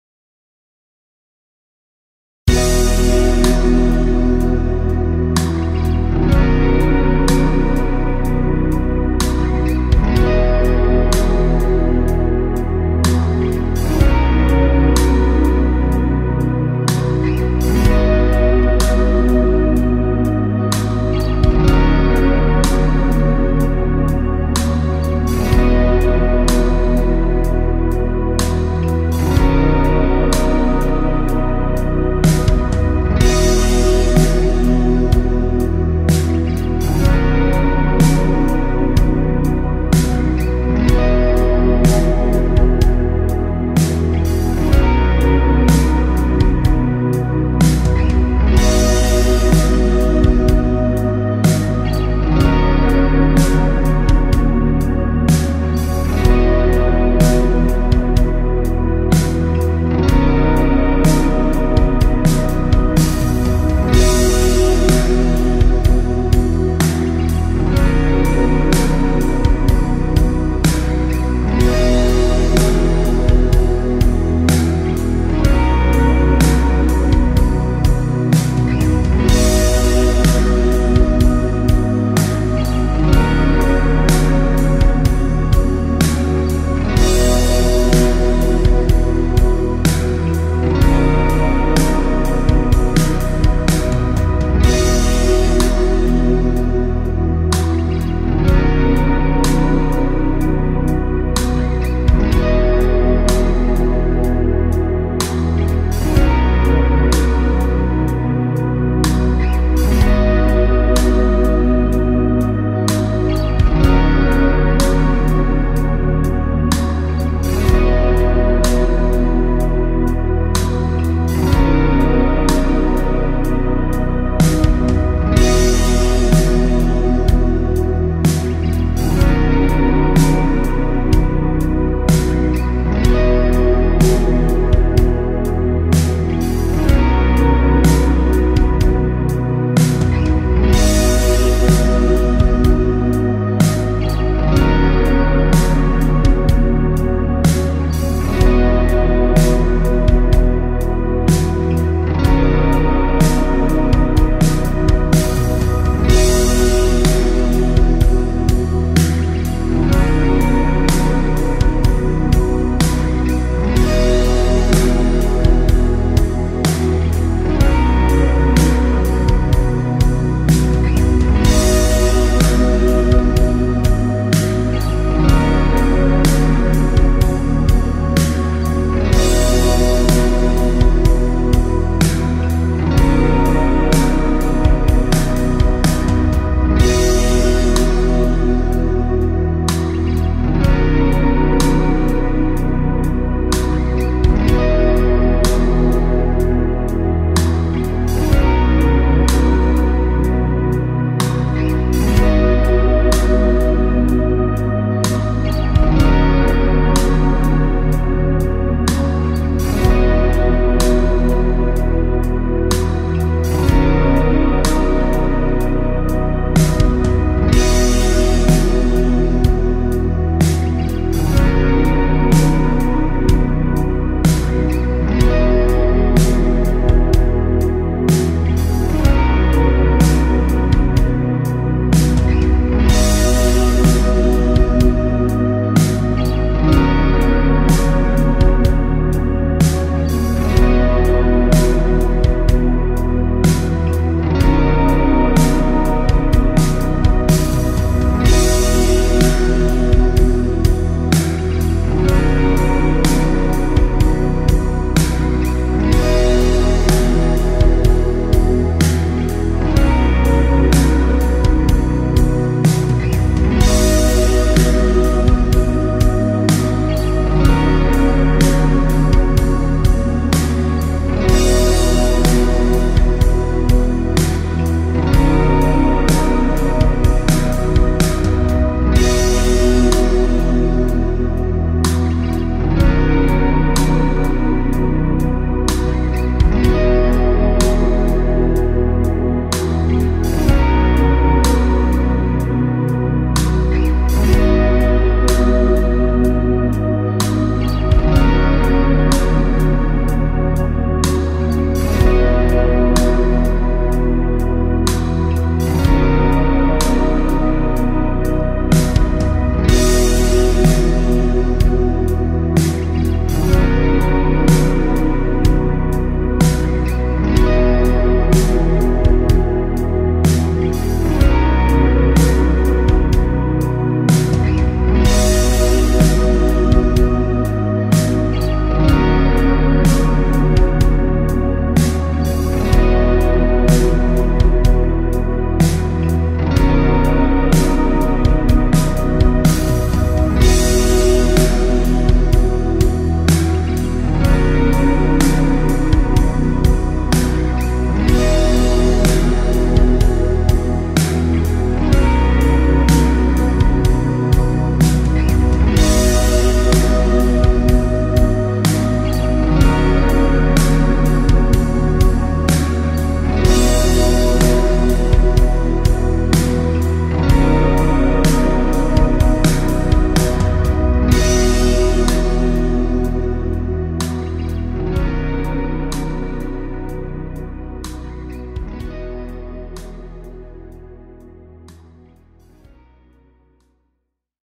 Zac Patrick YouTube Pop Ballad Backingtrack in B-Major
Hi everybody

Here is the Backingtrack for the Video i did with my moded Squier Affinity Strat from 1999.
It's in B-Major. I hope you have fun with it ;-)